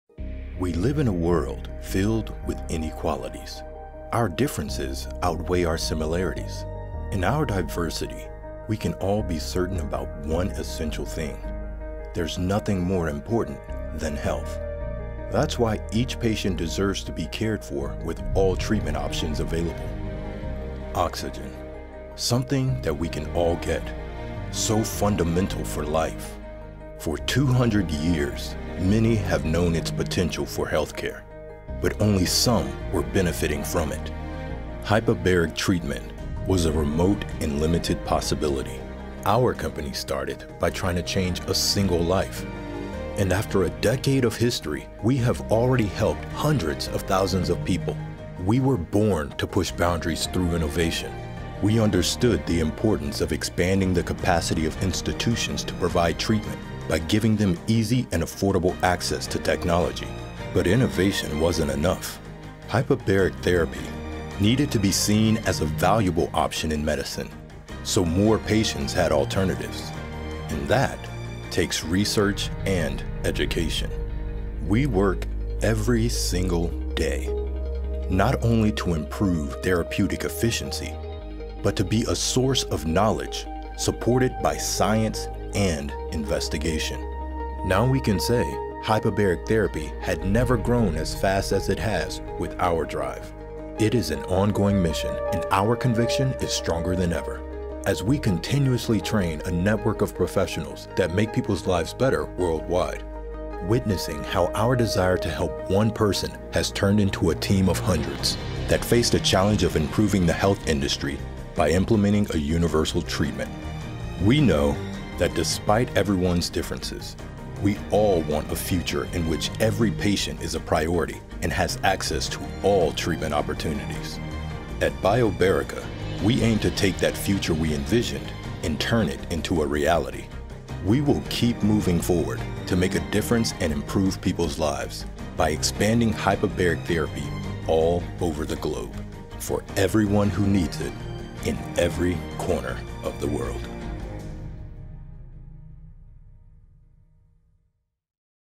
Our Purpose | Join us into the new era of healthcare
For more than a decade, Biobarica has worked to spread Hyperbaric Oxygenation Treatment and turn it into a real possibility for everyone who needs it. We are committed to developing technologies to make a difference in patient care through an efficient and safe therapy.
It is an ongoing mission and we will continue to push boundaries and expand hyperbaric therapy all over the globe.